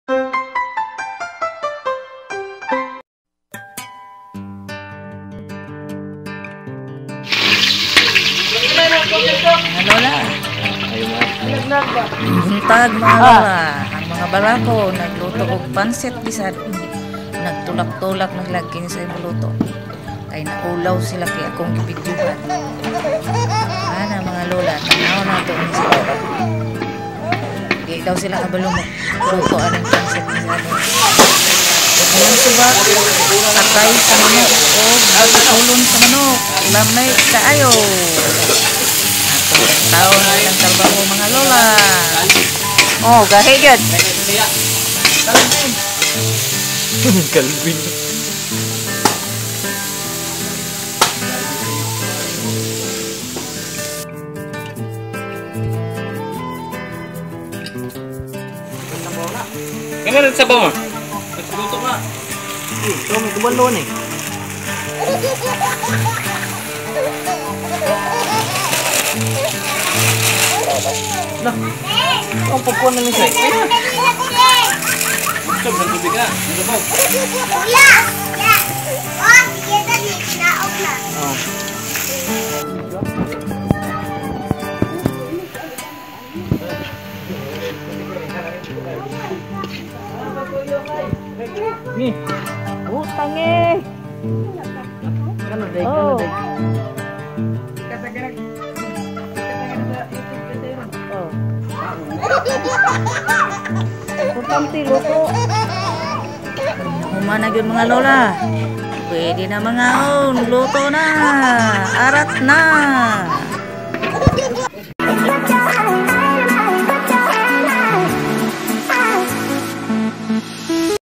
PANCIT guisado w/ ATAY at BATIKOLON ng manok luto ng mga BARAKO .
Pancit guisado
ingredients
atay ay batikolon mas marami mas masarap
oil ,sibuyas ,bawang,atsal,cabage,bagyo beans,carrot ,toyo,asin pancit paminta powder.
paraan sa pag luto
pakoloin ang mantika pag mainit na ilagay ang mgq rekado sabay sabay na yan ang ginaw ng mga barako sa pag luto nila pag golden brown na ilagay ang atay at batikolon ng manok lutoin ng maigi iaunod ang mga gulay at pancit lagyan ng asin at toyo at tubig halu haluin hangang maluto...